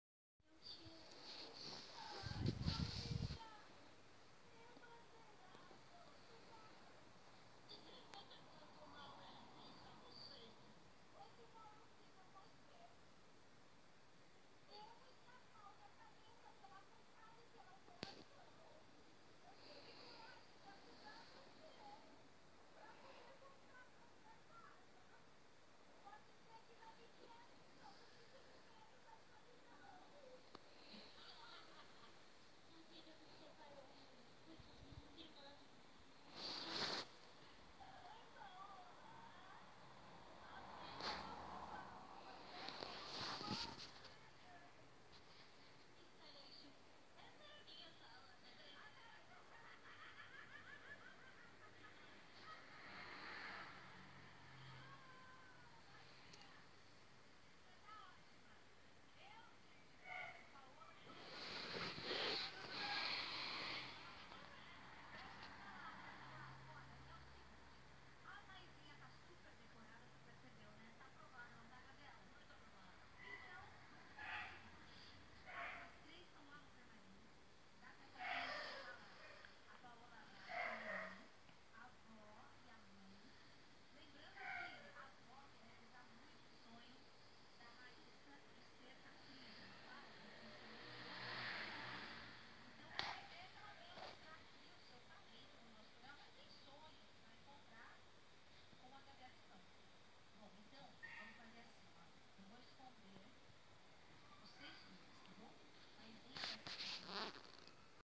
24 semanas de gestação (sabado) todo sábado faço a semana...
6 meses de gestação, mas gravei num. Domingo...